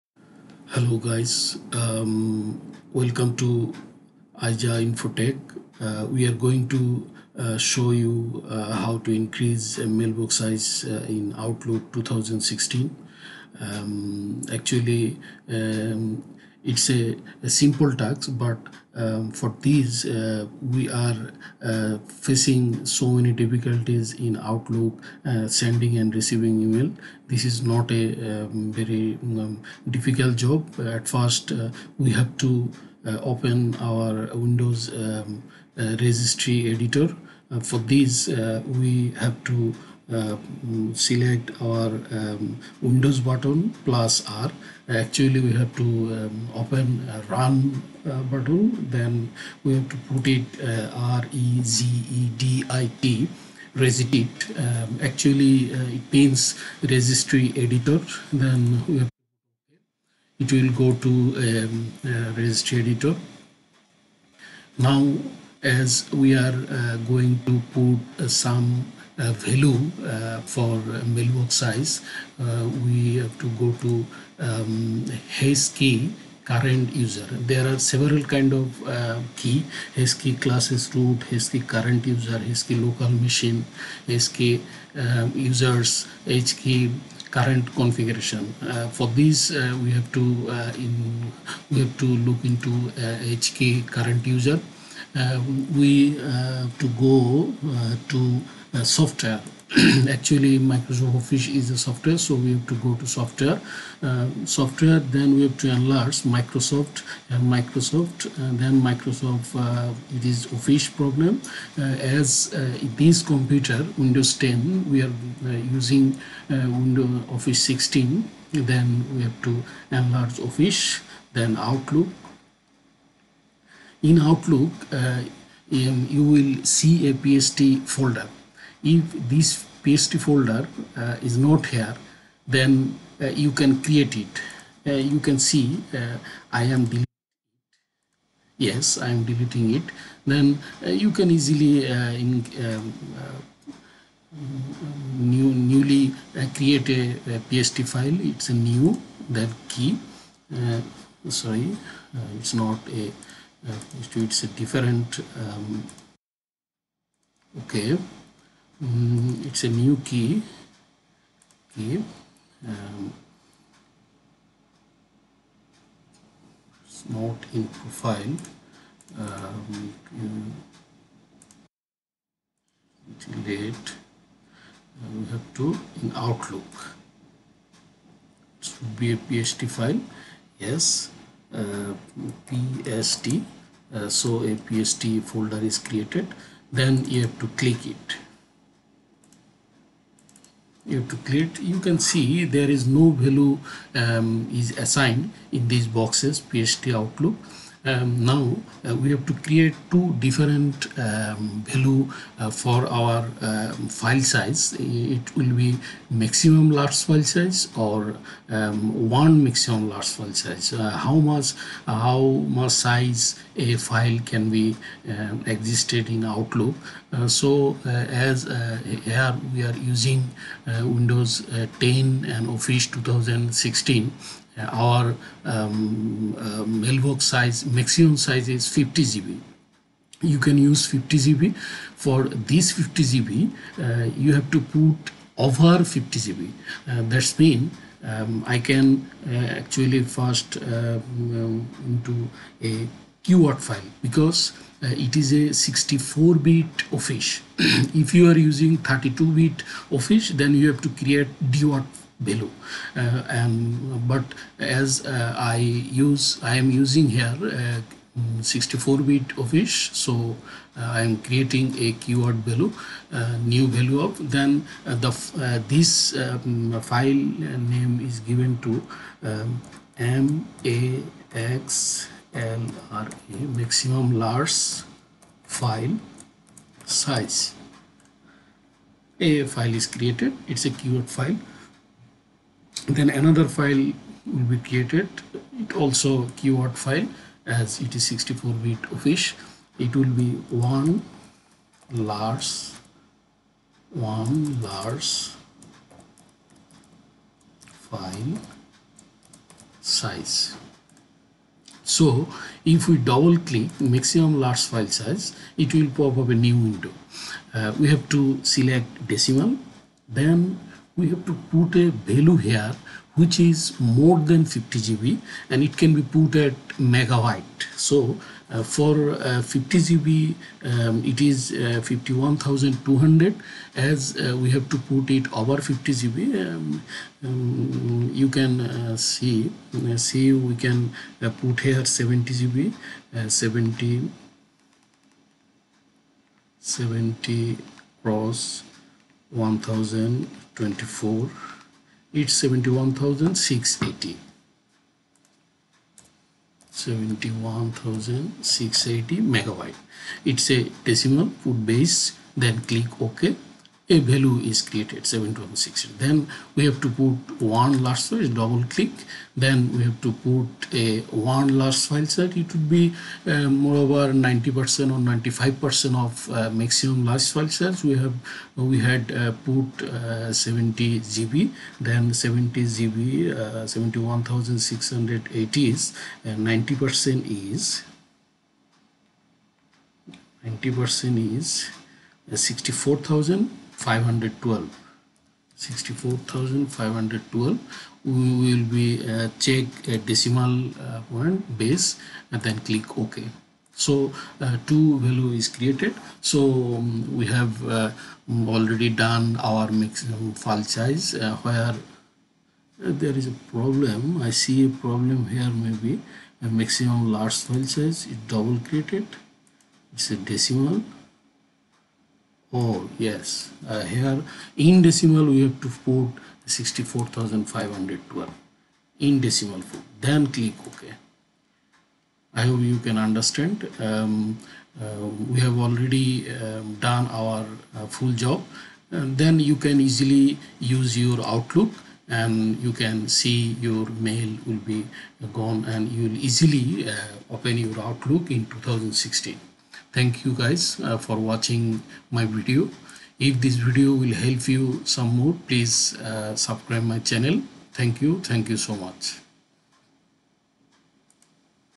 How to increase mailbox size of outlook 2016.... windows10 office #2016
It will show how you can increase your outlook mailbox size